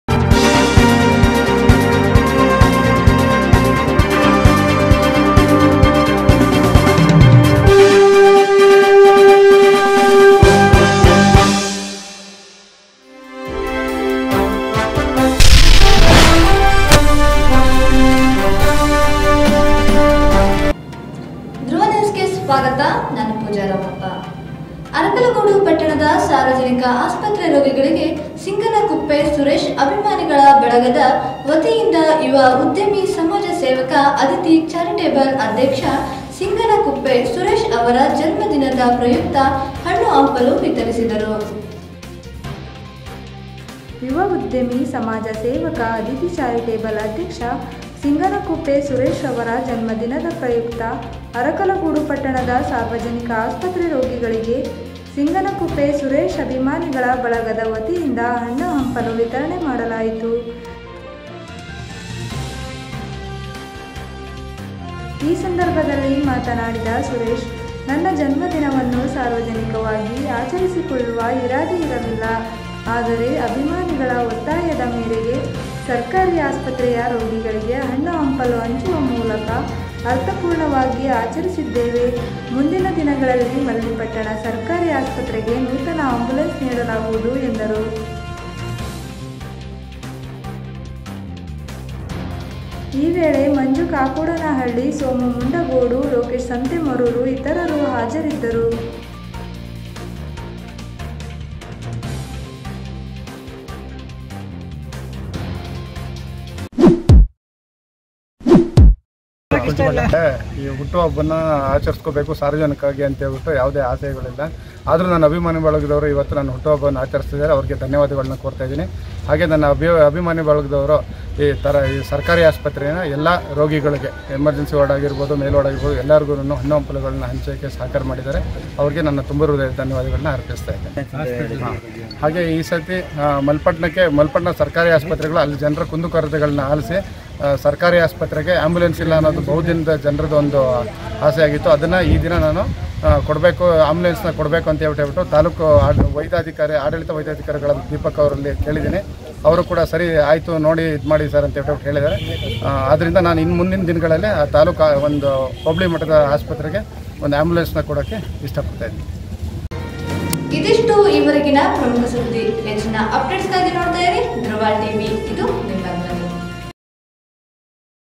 ಅರಕಲಗೂಡು ಪಟ್ಟಣದ ಸಾರ್ವಜನಿಕ ಆಸ್ಪತ್ರೆ ರೋಗಿಗಳಿಗೆ
ಅಧಿತಿ ಚಾರಿಟೇಬಲ್ ಅಧ್ಯಕ್ಷ ಸಿಂಗನಕುಪ್ಪೆ ಸುರೇಶ್ ರವರ ಜನ್ಮ ದಿನದ ಪ್ರಯುಕ್ತ ಹಣ್ಣುಹಂಪಲು ವಿತರಣೆ